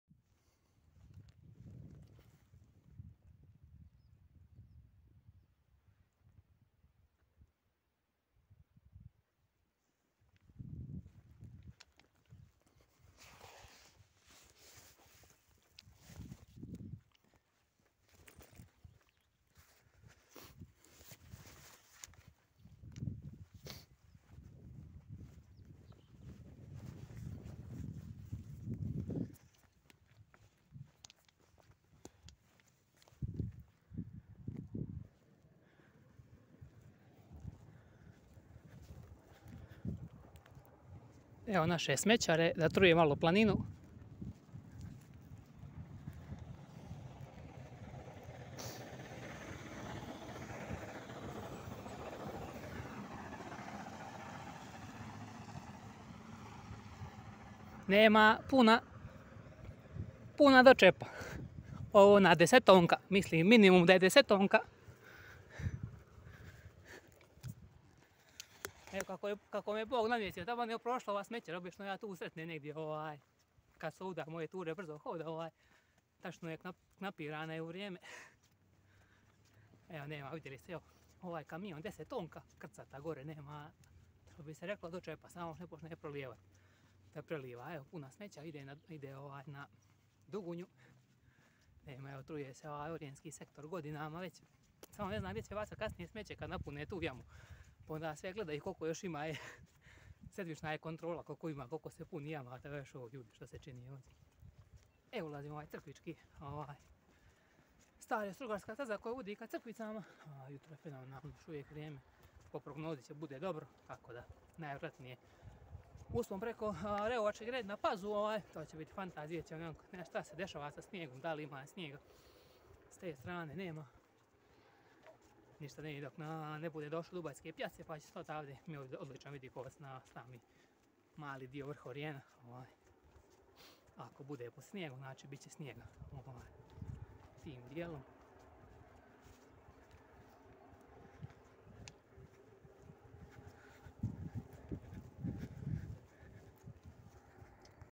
Proljećni brzi hod 65.5 km,uspon na Reovačku gredu vrh "Pazua 1769 mnv" 21.April 2023.
Via Crkvice